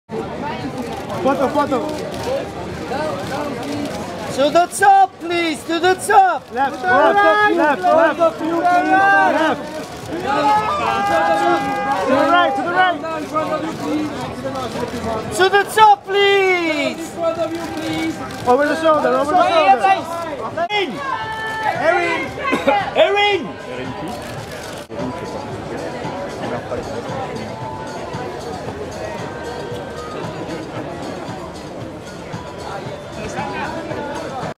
Erin Etherton at the Cavalli Boat party, Cannes
Erin Etherton, Leonardo Dicaprio's ex girlfriend at the Cavalli Boat party, Cannes

Cannes, France, May 22nd 2013